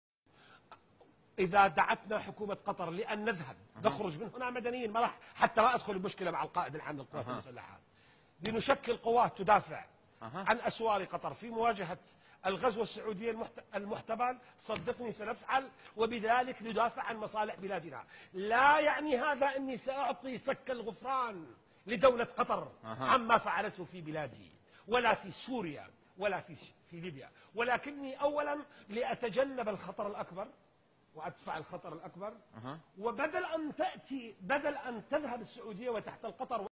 Iraqi MP al Juburi "Qatar is lesser of 2 evils - Saudi is bigger threat in region"
Iraqi MP Mishaan al Juburi argues that although he has his reservations over Qatar, he believes defending them from Saudi is more important. He says that sending troops to help defend Qatar from Saudi advances would be to the benefit of Iraq.